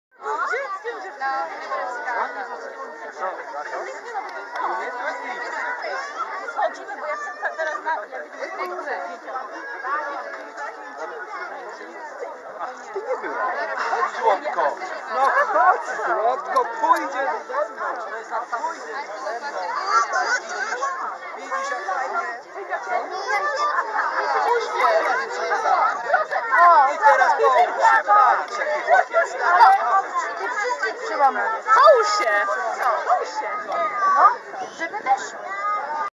Spacer w chmurach
W Sudetach w paśmie Śnieżnika. Na szczycie Karkonowskiej Góry w Czechach. Jest instalacja " Spacer w chmurach." Jej wysokość 50 m. Pod tą siatką na szczycie jest przepaść 50 metrowa. Niezapomniane wrażenia i...adrenalina.